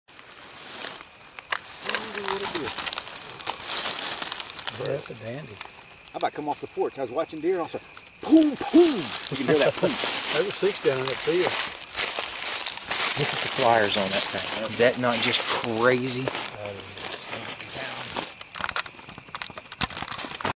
260 plus inch giant whitetail
Hunting giant bucks in southern Illinois.